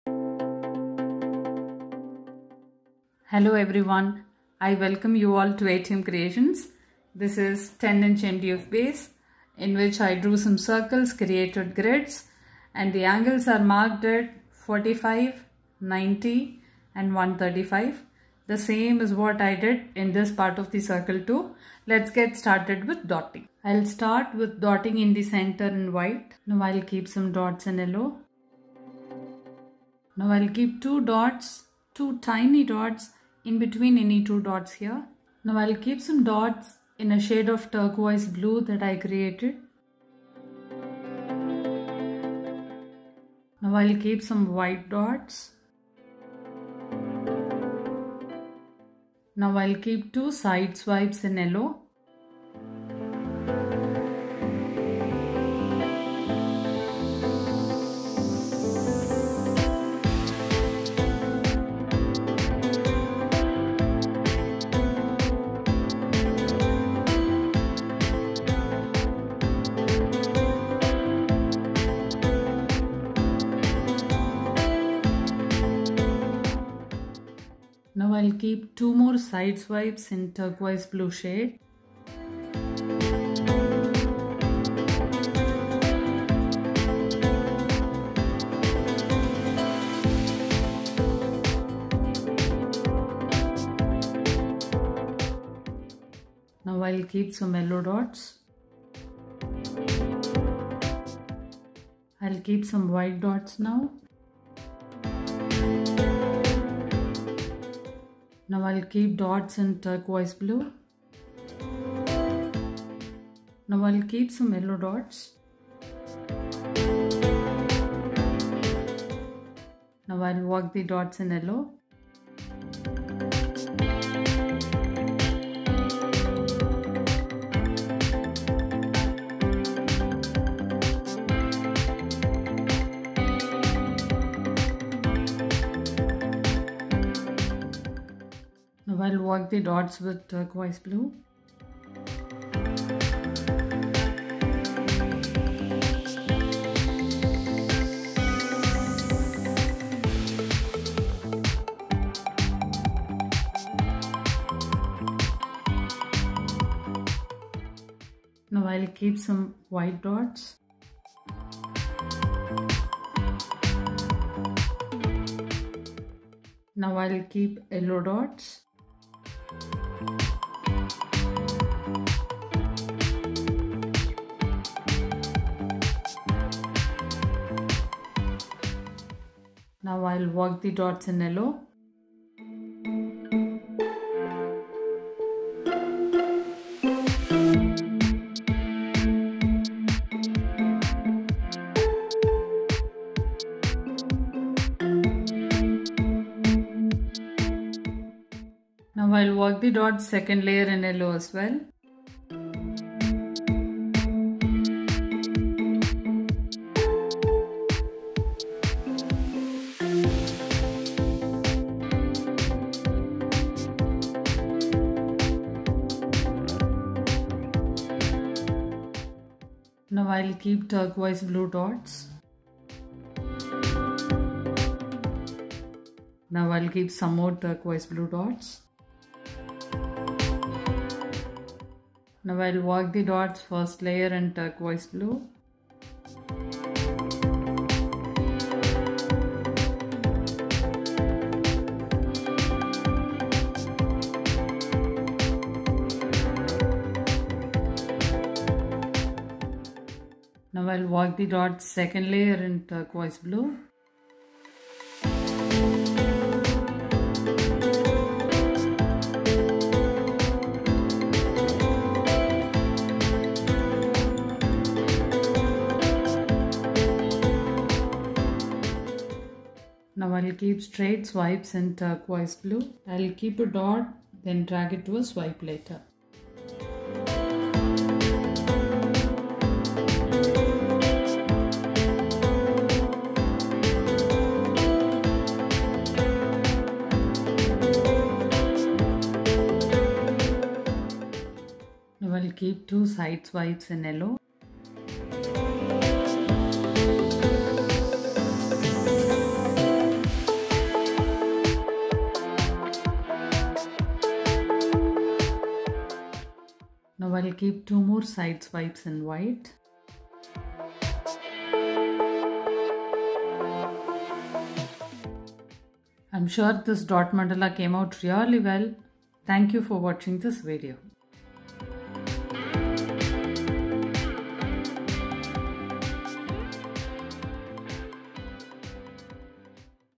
Dot Painting | Dot Art | Dot mandala for beginners | 122 | 2023 | ATM Creations
This is Dot mandala on MDF and a Dot Mandala for Beginners , explained Step by step. This is the 122nd dot mandala in the MDF series, that's uploaded in year 2023, by ATM Creations.

Created on a 10" MDF board of 2.5mm thickness